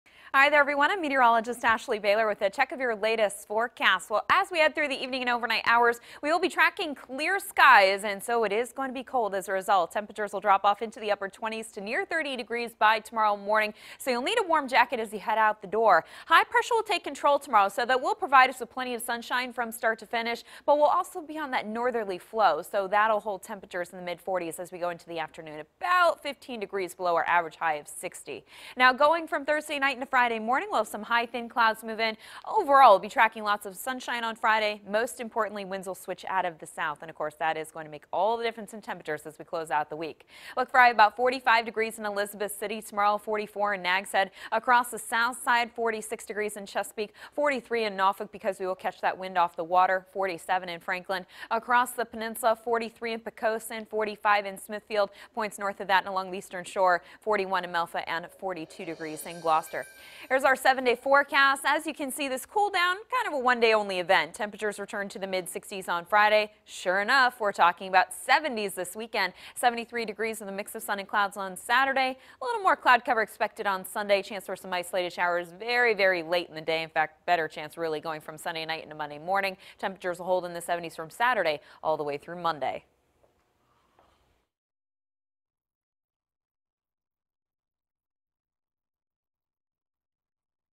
Super Doppler 10 Evening Forecast
Meteorologist Ashley Baylor's evening forecast for March 22, 2017.